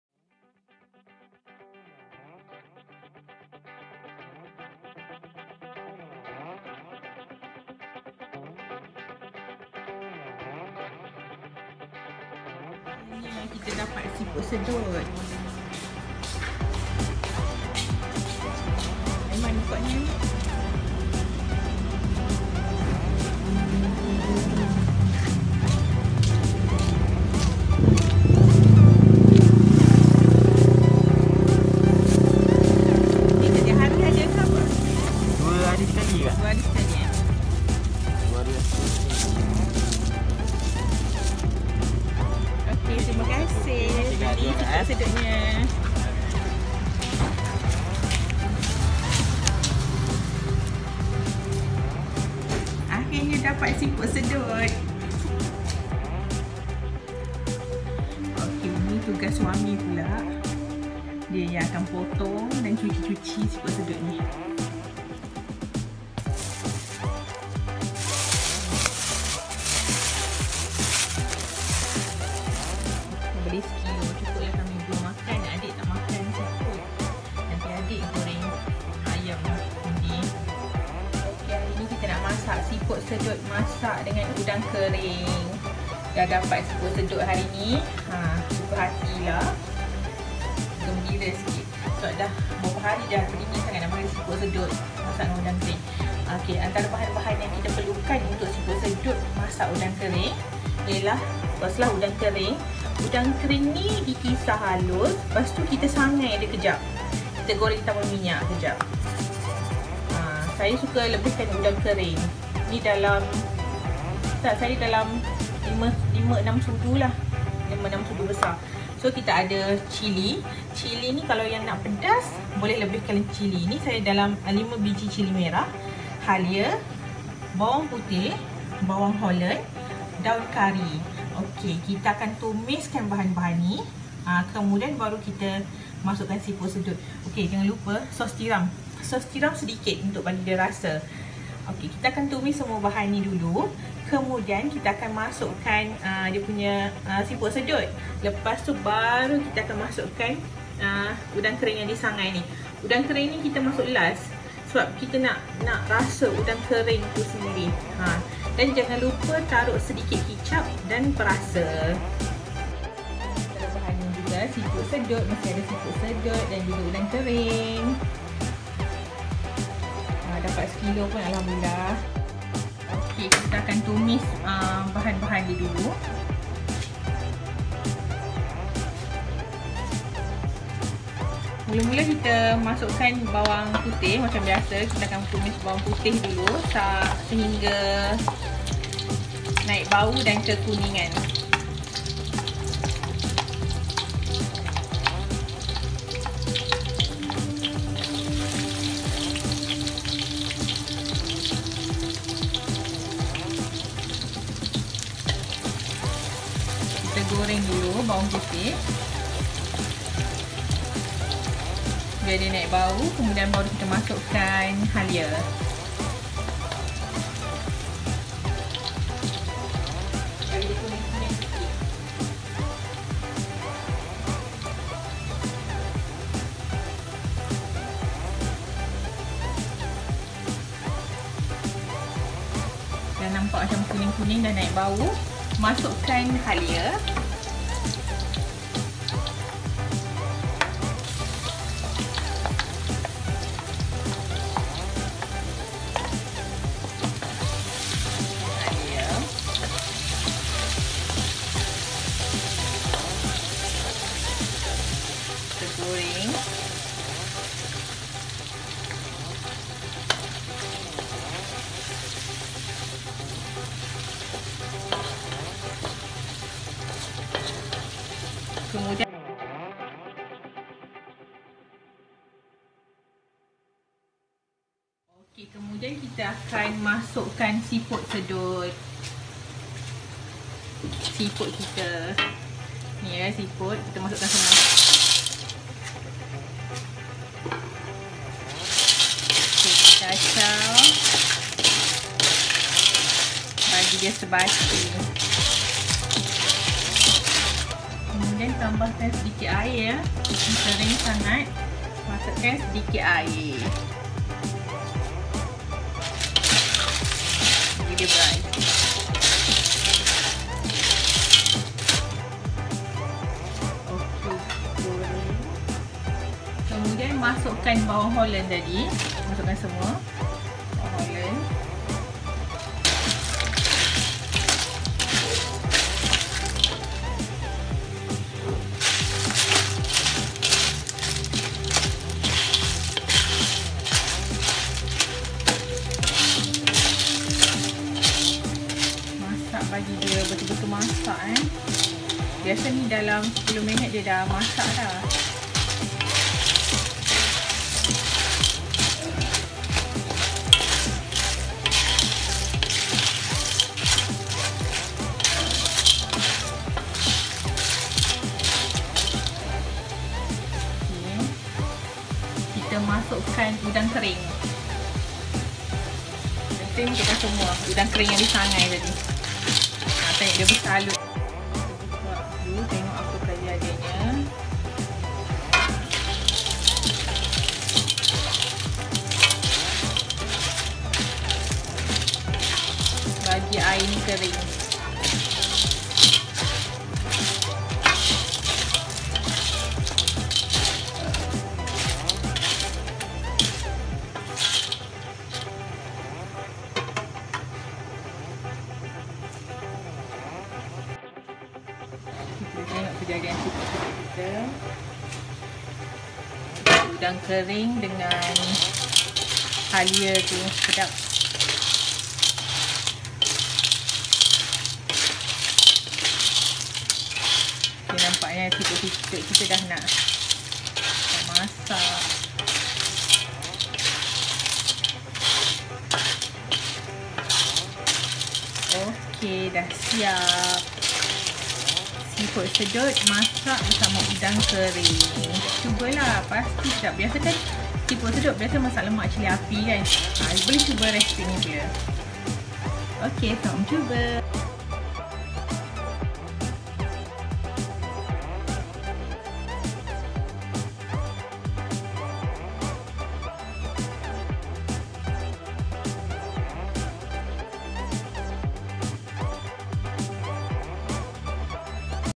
Siput Masak Udang Kering
lebihkan udang kering untuk lebih sedap. Jika nak pedas lebihkan cili. Garam kemudiankan kerana sos tiram & udang kering dah masin tau.
Selamat Mencuba